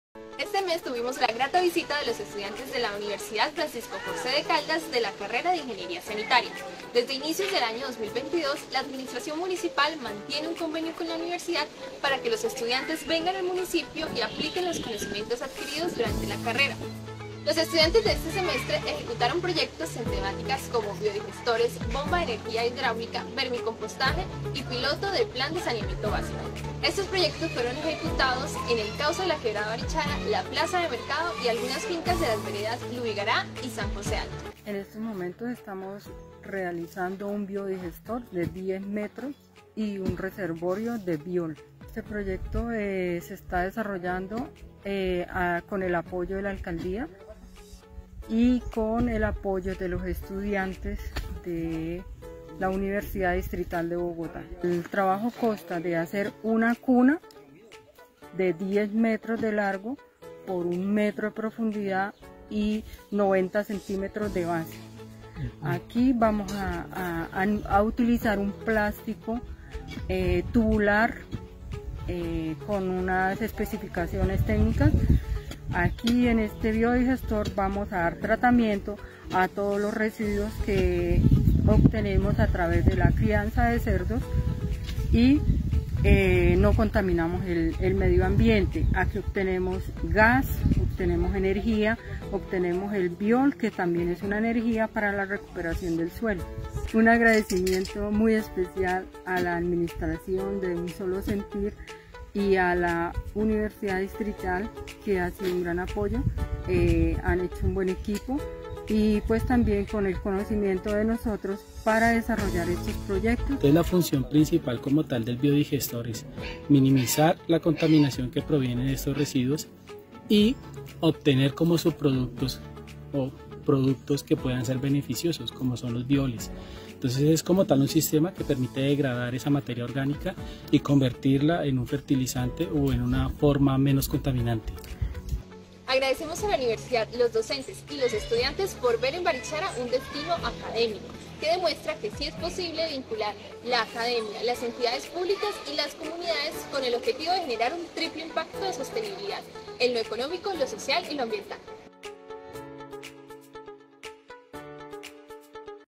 UNIVERSIDAD FRANCISCO JOSÉ DE CALDAS VISITA BARICHARA 2023
Un grupo de estudiantes de Ingeniería Sanitaria estuvieron aplicando sus conocimientos en biodigestores, piloto de plan de saneamiento básico, entre otros...